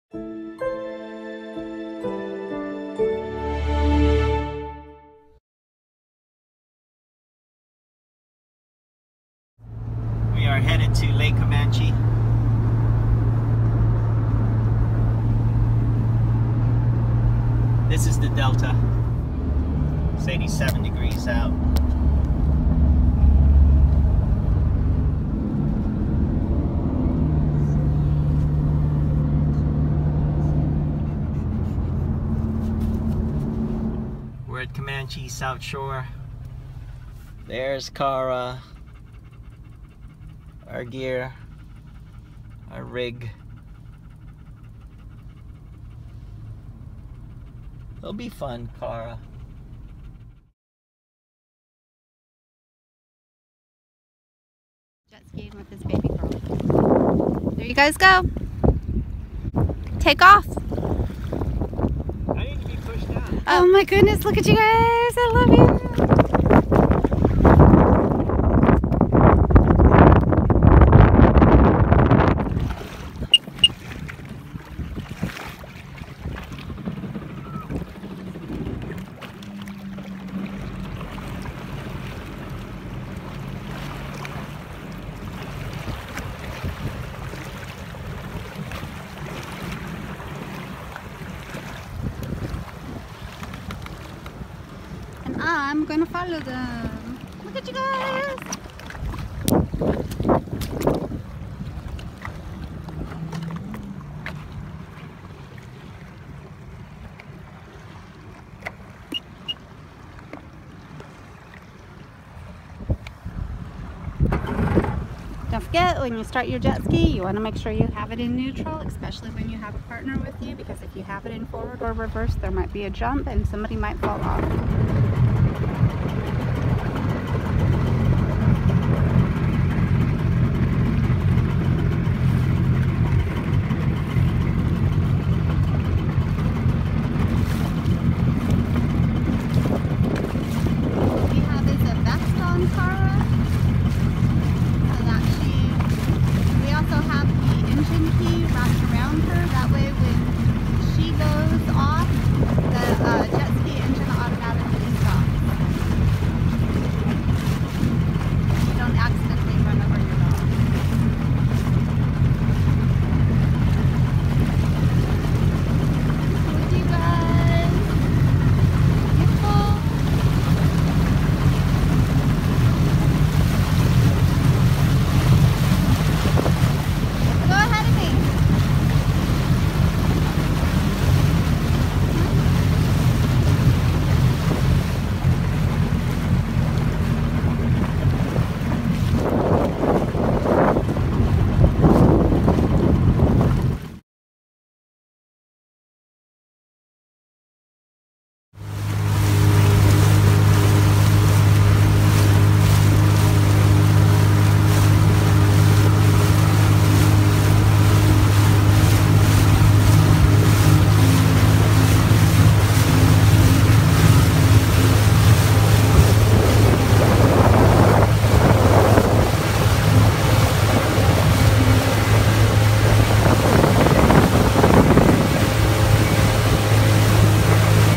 German Shepherd riding seadoo jet ski. Please read description 1 of 4 Videos Series
Happy Father's Day!  1 of 4 Videos Series
Kara German Shepherd riding seadoo jet ski.
This is her first Lake and Seadoo experience! She did amazing! We throttled down a lot for Kara! No rough riding. We made it as comfortable as we can for Kara (no jumping, no sharp turn, etc).
We want her to do this again as this is one of our favorite summer activities.
GSDs are very smart. They will not do anything that they dislike (with strong protest if forced).
Wife or I stayed at base camp with Kara so one of us could ride our seadoo (solo) full throttle to get our speed - adrenaline fix. I am very happy Kara enjoyed riding on it (not fast). In the future, we bet we could go faster if water is "glass". Yes, she is wearing a floatation device "vest" designed for dogs. Safety first for our baby!
There are a lot of things my wife and I did but stopped due to Kara. We are slowly trying to figure out... how we can include Kara on all of them. This is one of those things.
Dog riding seadoo
Dog riding jet ski